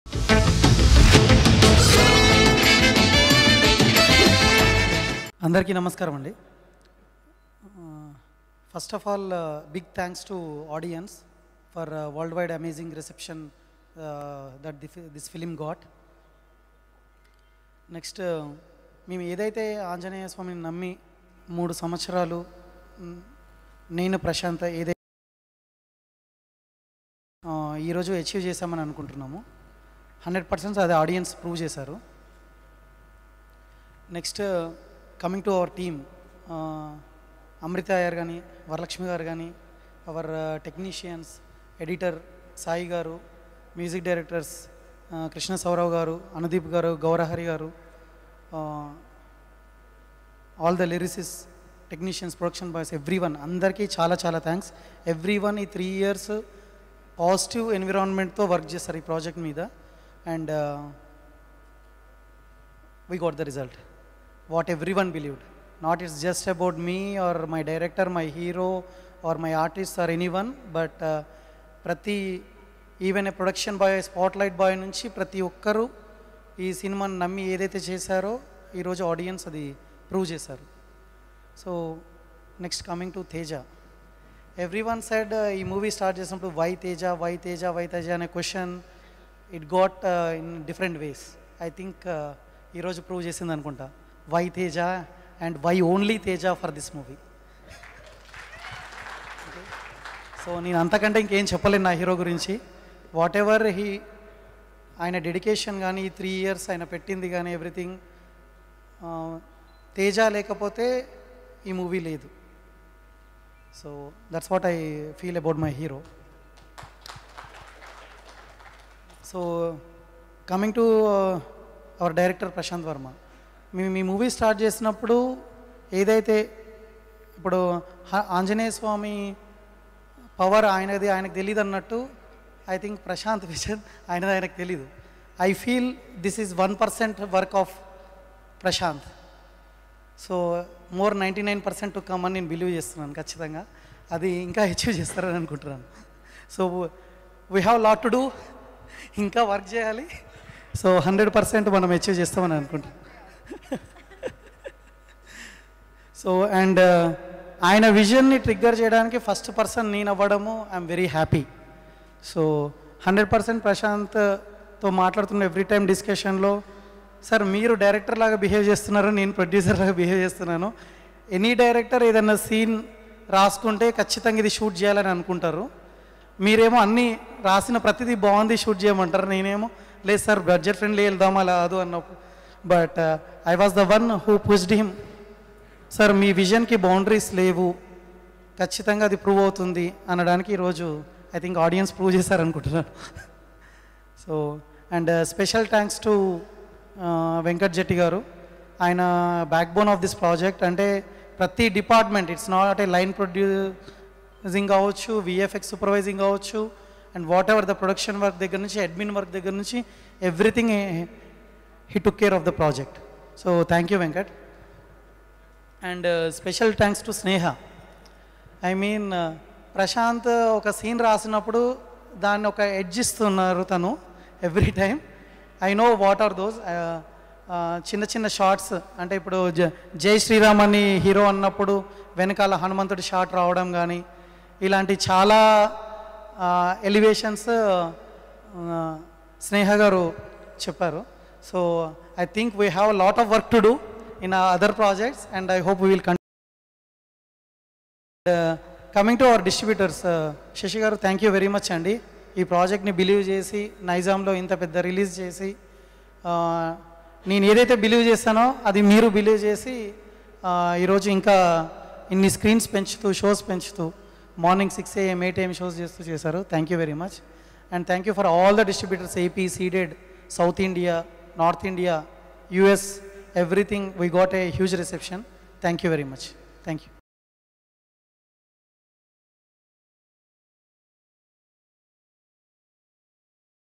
Producer Niranjan Reddy Speech @ HANUMAN Gratitude Meet | Teja Sajja | Prasanth Varma
Watch Producer Niranjan Reddy Speech @ HANUMAN Gratitude Meet.